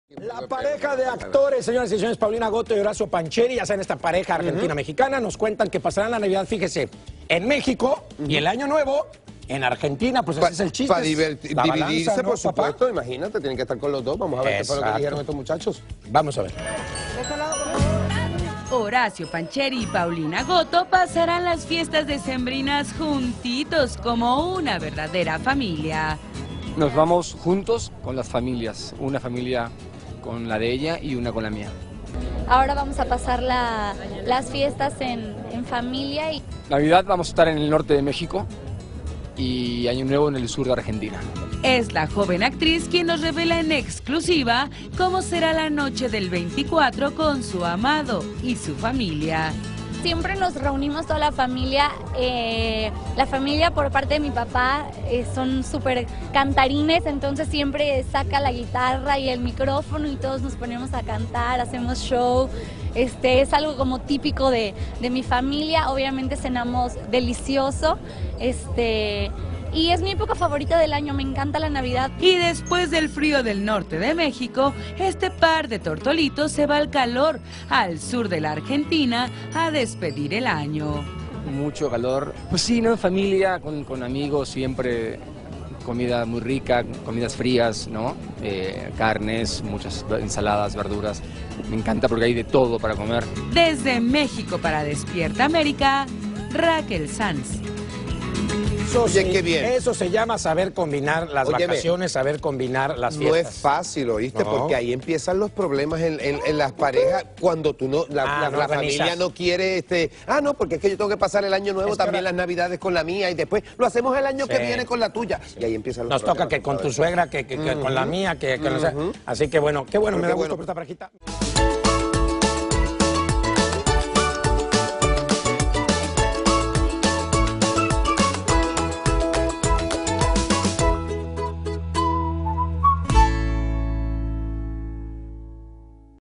Paulina Goto y Horacio Pancheri recorrerán casi todo el continente esta Navidad
La pareja de actores confirma que pasarán las Navidades tanto con la familia de Paulina en México, como con la familia de Horacio en Argentina.